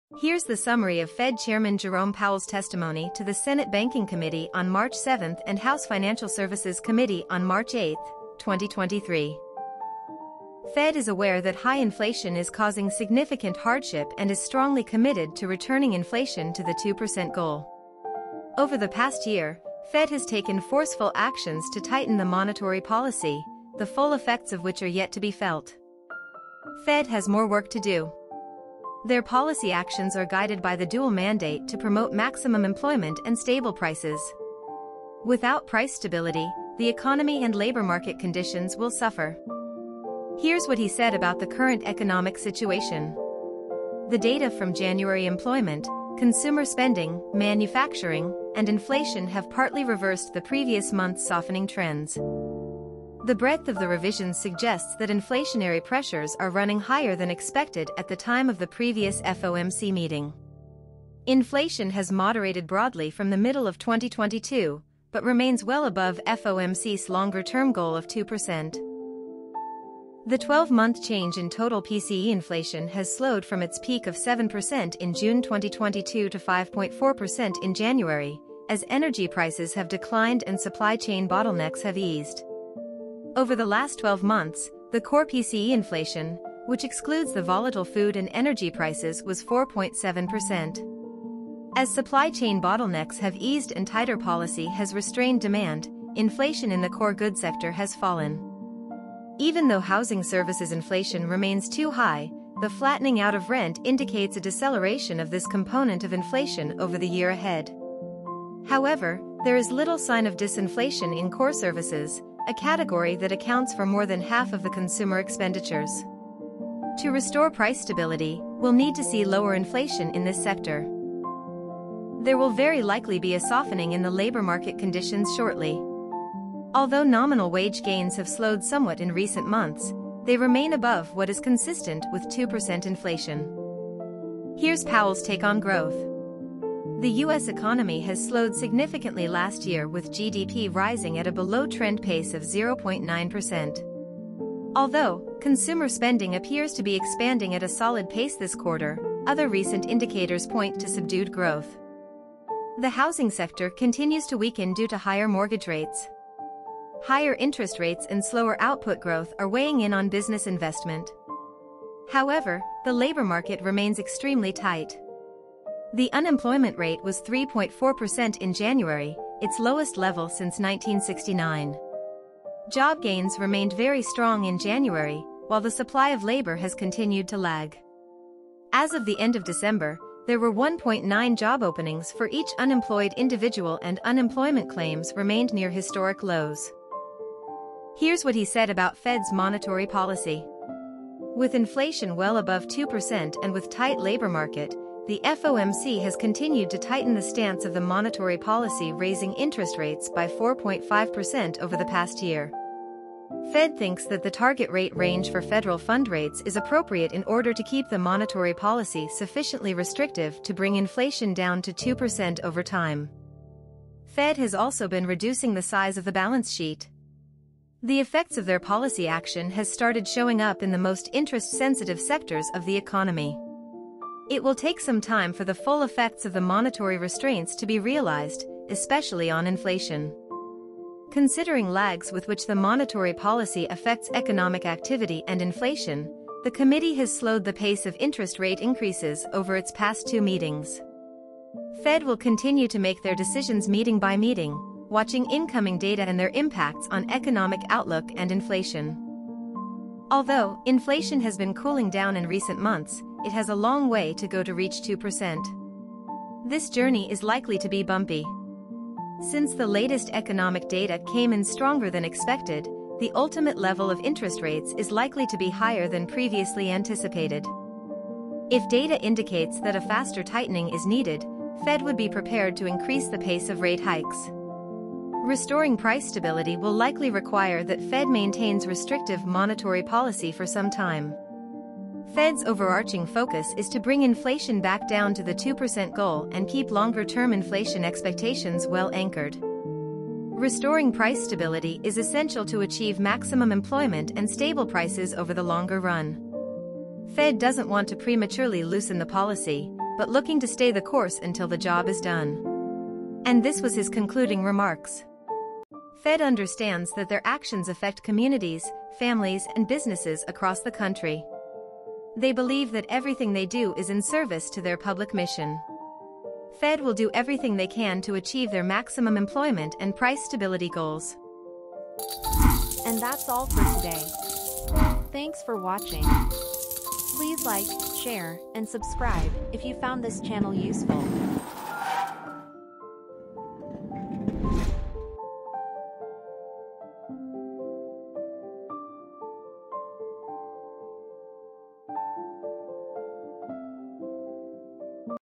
SUMMARY-Fed Chair Jerome Powell’s Testimony to US Congress (7th & 8th March, 2023) | Grow Your Money
Here’s the summary of Fed chairman Jerome Powell’s speech at the Senate Banking Committee meeting on March 7th and House Financial Services Committee on March 8th, 2023.
Fed is aware that high inflation is causing significant hardship and is strongly committed to returning inflation to the 2% goal.
Over the past year Fed has taken forceful actions to tighten the monitory policy, the full effects of the which are yet to be felt.
Fed has more work to do.
Their policy actions are guided by the dual mandate to promote maximum employment and stable prices.
Without price stability, the economy and labor market conditions will suffer.
Current Economic Situation:
The data from January on employment, consumer spending, manufacturing production, and inflation have partly reversed last month’s softening trends.
The breadth of the revisions suggests that inflationary pressures are running higher than expected at the time of the previous FOMC meeting.
Inflation has moderated broadly from the middle of 2022, but remains well above FOMC’s longer term goal of 2%.
The 12 month change in total PCE inflation has slowed from its peak of 7% in June 2022 to 5.4% in January, as energy prices have declined and supply chain bottlenecks have eased.
Over the last 12 months, the core PCE inflation, which excludes the volatile food and energy prices was 4.7%
As supply chain bottlenecks have eased and tighter policy has restrained demand, inflation in the core good sector has fallen.
Even though housing services inflation remains too high, the flattening out of rent indicates a deceleration of this component of inflation over the year ahead.
However, there is little sign of disinflation in core services – a category that accounts for more than half of the consumer expenditures. To restore price stability, we’ll need to see lower inflation in this sector.
There will very likely be a softening in the labor market conditions shortly.
Although nominal wage gains have slowed somewhat in recent months, they remain above what is consistent with 2% inflation.
Growth:
The US economy has slowed significantly last year with GDP rising at a below trend pace of 0.9%.
Although, consumer spending appears to be expanding at a solid pace this quarter, other recent indicators point to subdued growth.
The housing sector continues to weaken due to higher mortgage rates.
Higher interest rates and slower output growth are weighing in on business investment.
However, the labor market remains extremely tight
The unemployment rate was 3.4% in January, it’s lowest level since 1969.
Job gains remained very strong in January, while the supply of labor has continued to lag.
As of the end of December, there were 1.9 job openings for each unemployed individual and unemployment claims remained near historic lows.
Fed’s Monitory Policy:
With inflation well above 2% and with tight labor market, the FOMC has continued to tighten the stance of the monitory policy raising interest rates by 4.5% over the past year.
Fed thinks that the target rate range for federal fund rates is appropriate in order to keep the monitory policy sufficiently restrictive to bring inflation down to 2% over time.
Fed has also been reducing the size of the balance sheet.
The effects of their policy action has started showing up in the most interest sensitive sectors of the economy.
It will take some time for the full effects of the monitory restraints to be realized, especially on inflation.
Considering lags with which the monitory policy effects economic activity and inflation, the committee has slowed the pace of interest rate increases over its past 2 meetings.
Fed will continue to make their decisions meeting-by-meeting watching incoming data and their impacts on economic outlook and inflation.
Although inflation has been cooling down in recent months, it has a long way to go to reach 2%. This journey is likely to be bumpy.
Since the latest economic data came in stronger than expected the ultimate level of interest rates is likely to be higher than previously anticipated.
If data indicates that a faster tightening is needed, Fed would be prepared to increase the pace of rate hikes.
Restoring price stability will likely require that Fed maintains restrictive monitory policy for some time.
Fed’s overarching focus is to bring inflation back down to 2% goal and keep longer term inflation expectations well anchored.
Restoring price stability is essential to achieve maximum employment and stable prices over the longer run.
Fed doesn’t want to prematurely loosen the policy, but looking to stay the course until the job is done.
Powell's Concluding Remarks:
Fed understands that their actions affect communities, families and businesses across the country.
They believe that everything they do is in service to their public mission.
Fed will do everything they can to achieve their maximum employment and price stability goals.